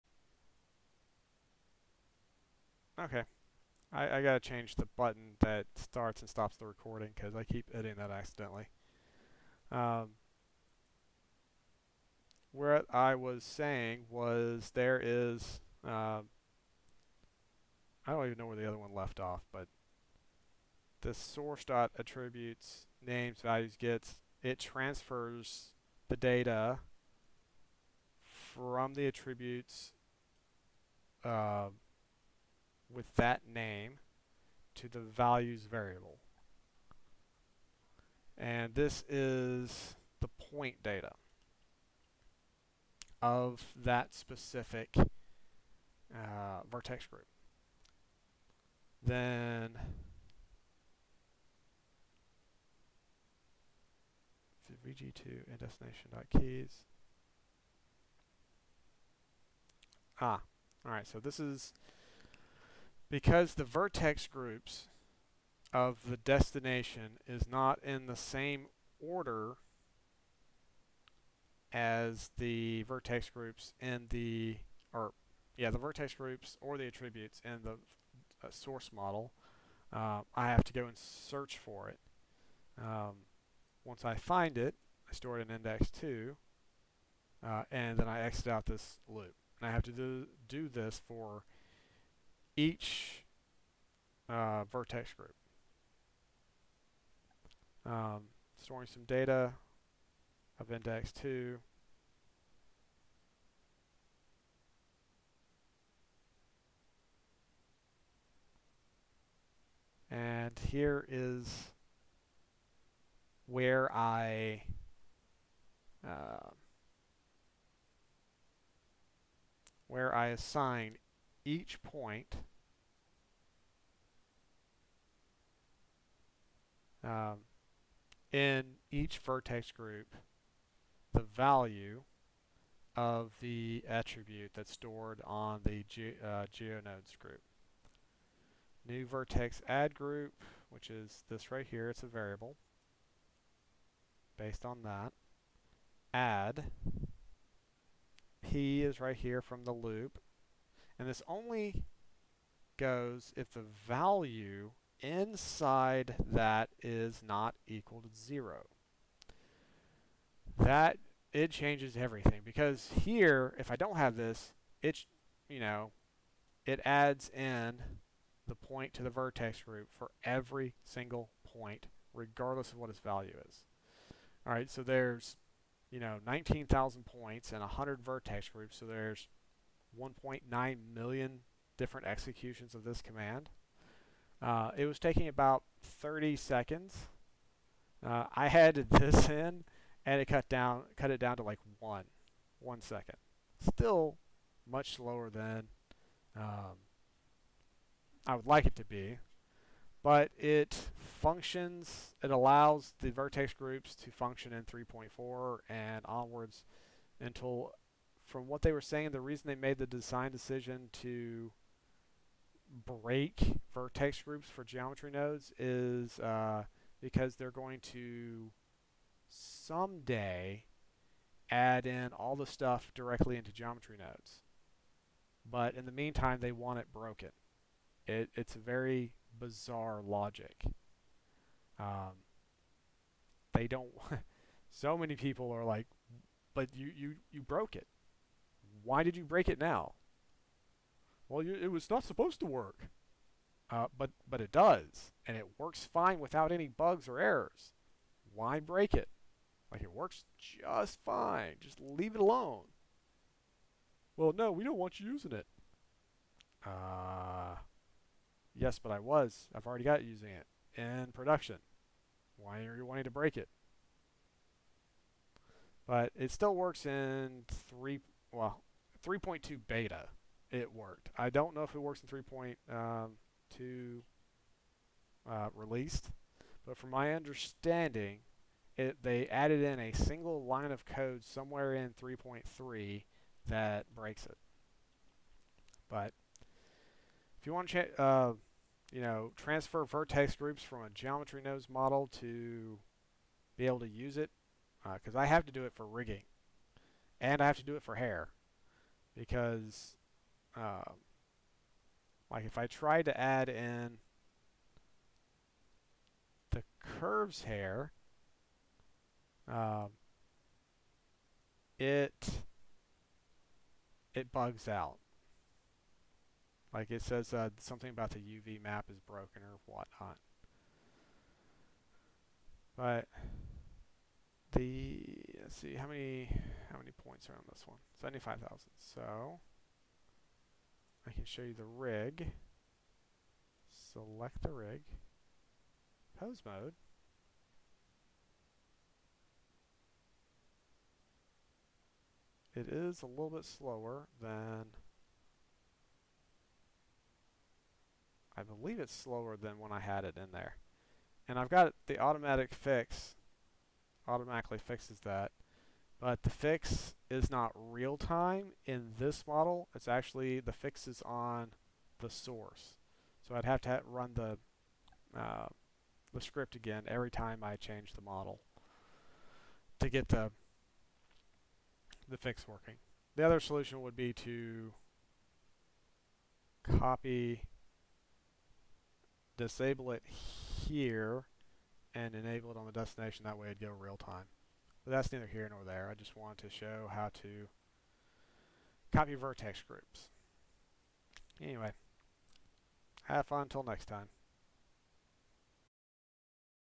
Blender Unleashed: Using Python to transfer Vertex data
Transferring mesh and vertex groups from geo-nodes created object to standard object